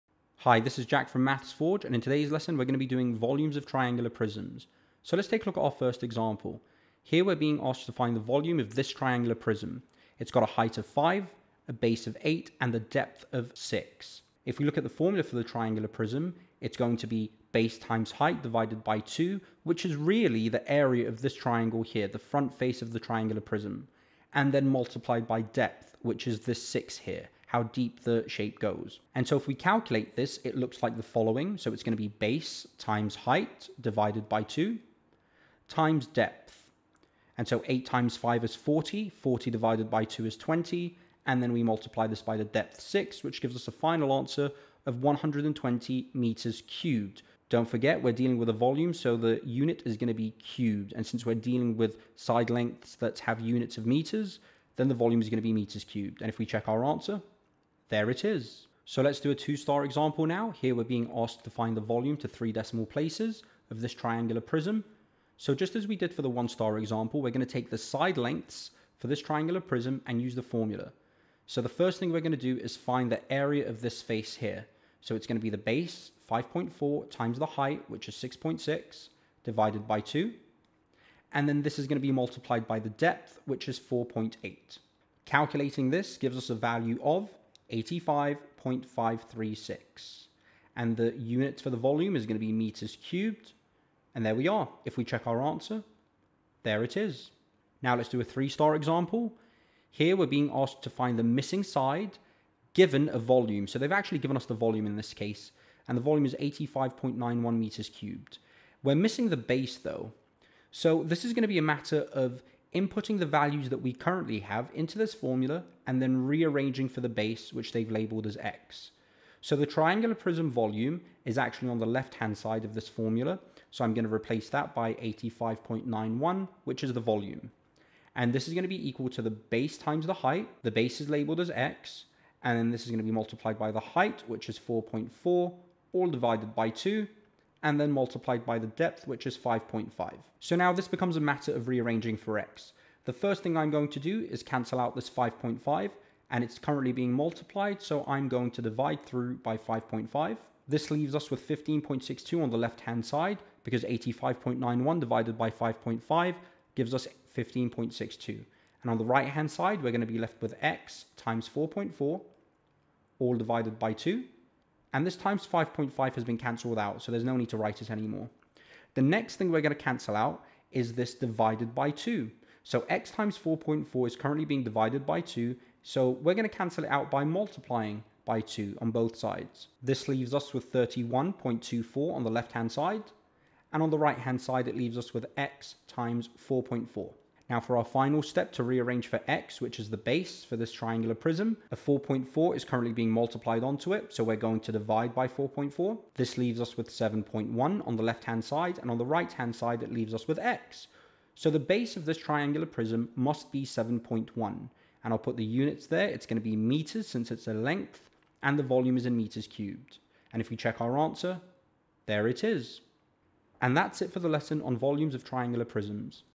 Volume (Triangular Prisms) - Geometry
Maths Forge brings you a lesson on how to find the volume of triangular prisms.

Triangle prism,
Volumes,
Triangular prism,
Triangular prisms,
Prism,
Prisms,
Volume,